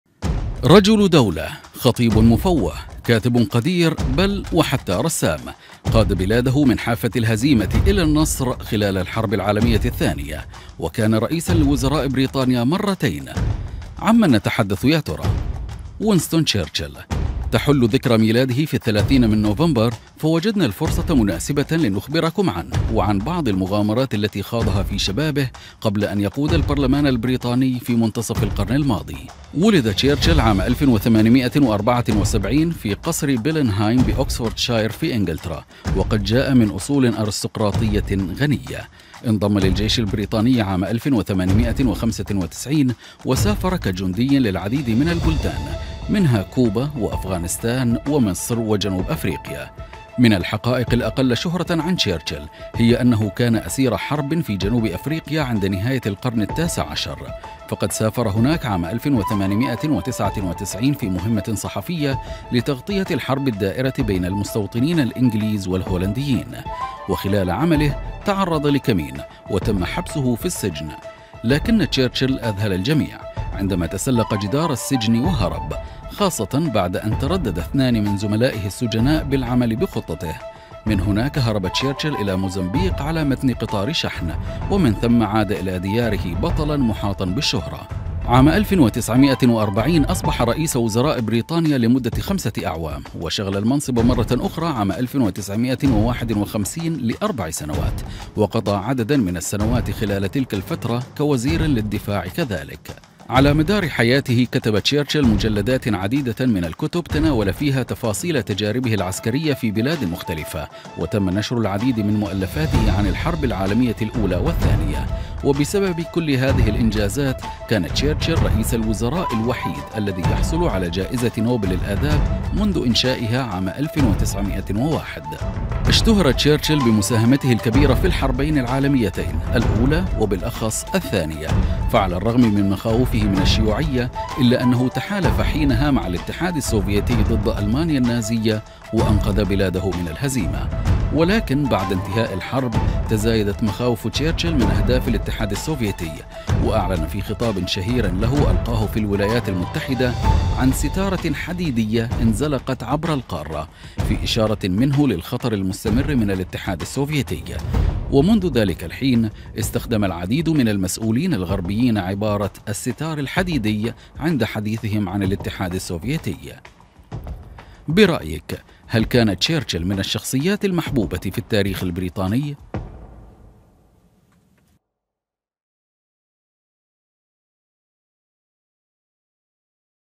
كيف هرب تشرشل من السجن؟
الكثير منا قد يعرف أن ونستون تشرشل كان رئيساً لوزراء بريطانيا في القرن الماضي إلا أن الفيديو القادم سيعرض لكم بعض المغامرات التي خاضها تشرشل في شبابه ومعلومات في الغالب أنكم لم تسمعوها من قبل..